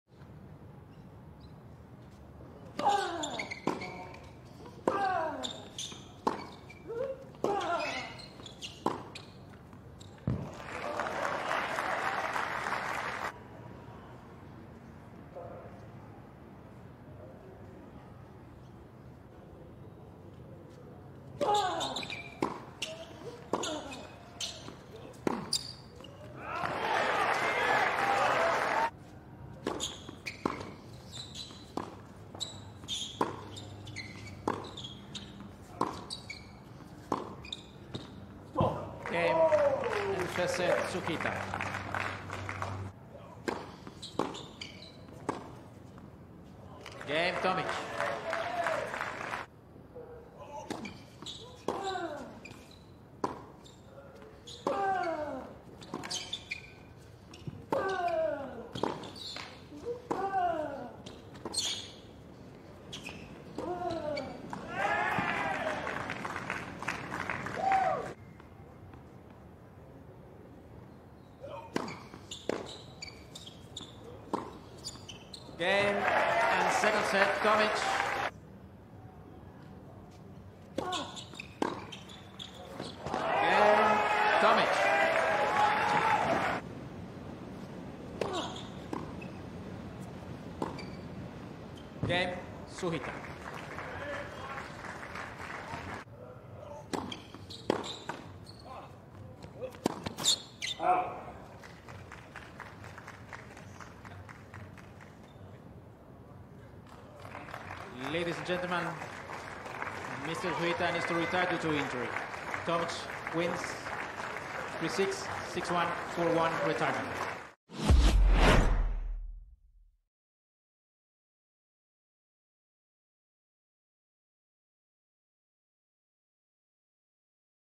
Yuichi Sugita vs Bernard Tomic match highlights (1R) | Australian Open 2021
Yuichi Sugita takes on Bernard Tomic in Round 1 of the Australian Open 2021.